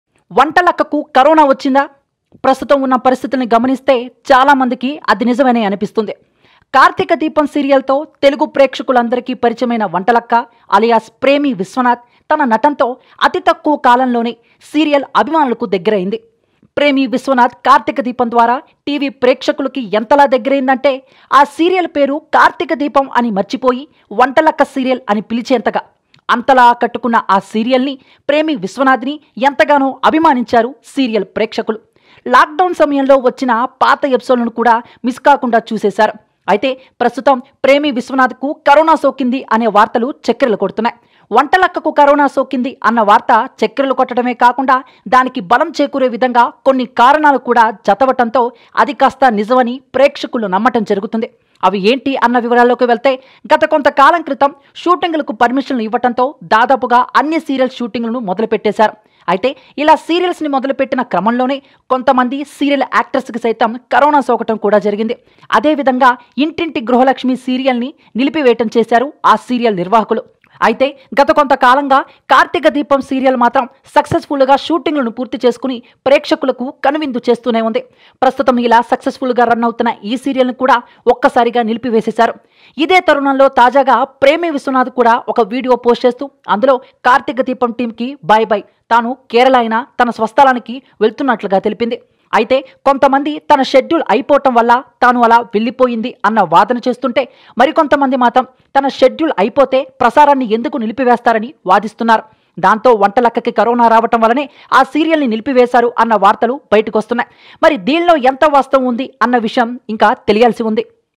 వంటలక్కకు కఱొనా పాజిటివ్: Serial Actress Deepa Tested Positive | News Buzz
వంటలక్కకు కఱొనా పాజిటివ్: Serial Actress Deepa Tested Positive